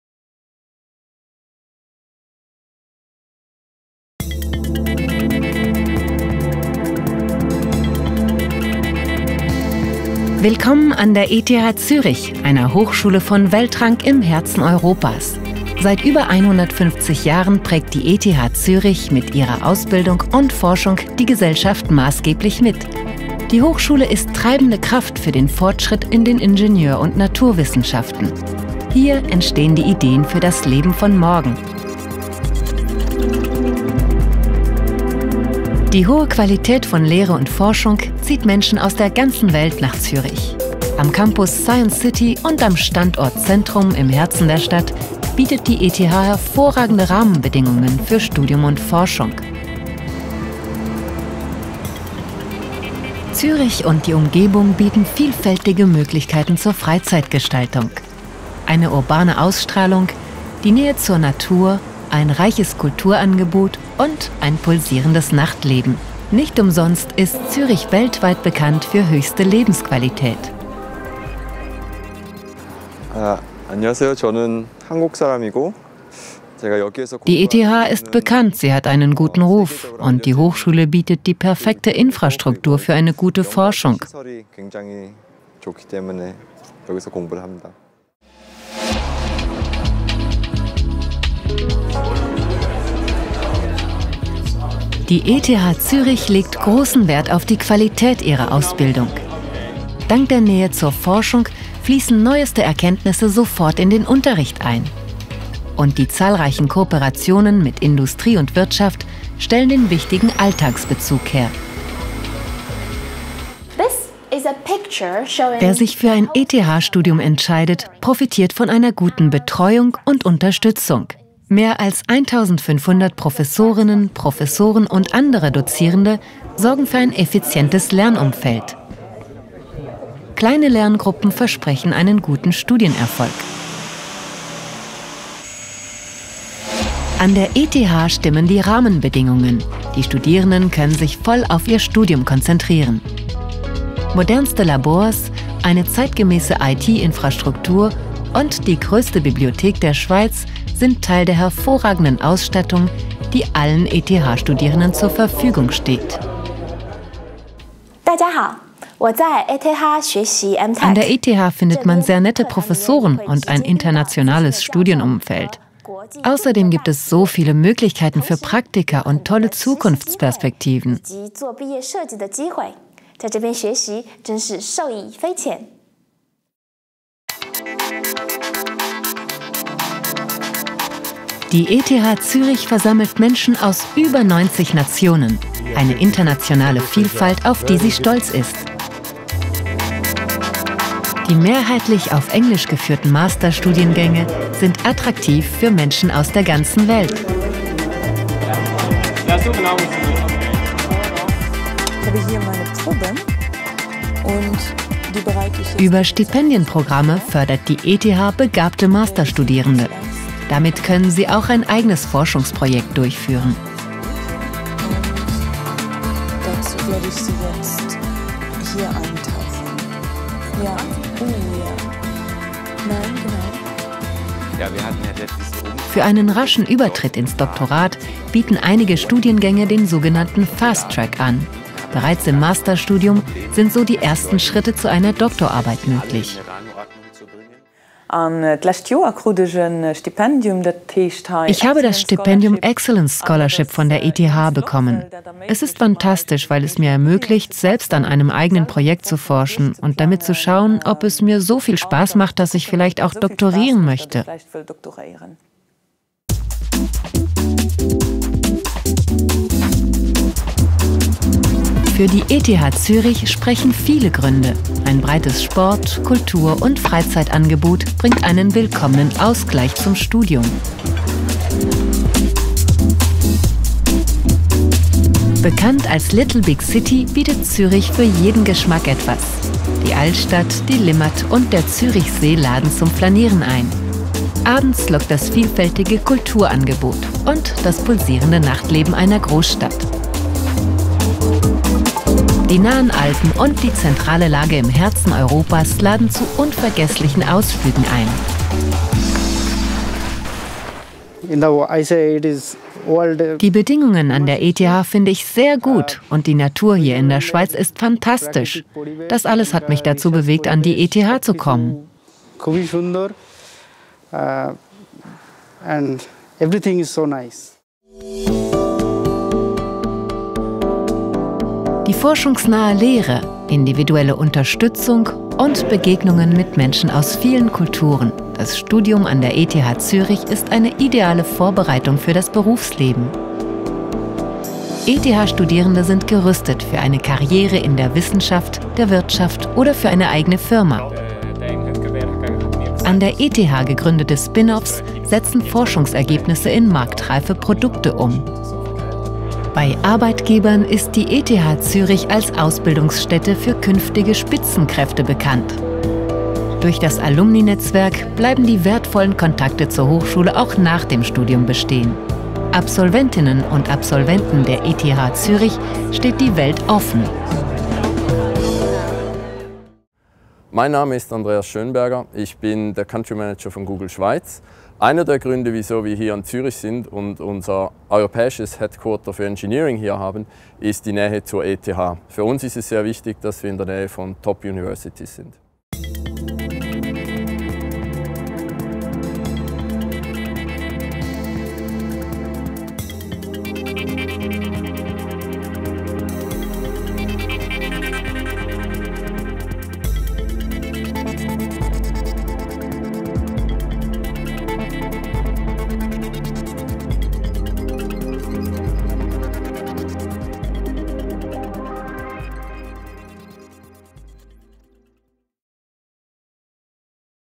Visual Tour: Studieren an der ETH Zürich
Die "Visual Tour" portraitiert die Lehre an der ETH Zürich, das Leben in Zürich sowie Karrieremöglichkeiten nach dem Studienabschluss. Jedes der 5 Video-Module konzentriert sich dabei auch einen ausgewählten Aspekt des ETH-Studiums.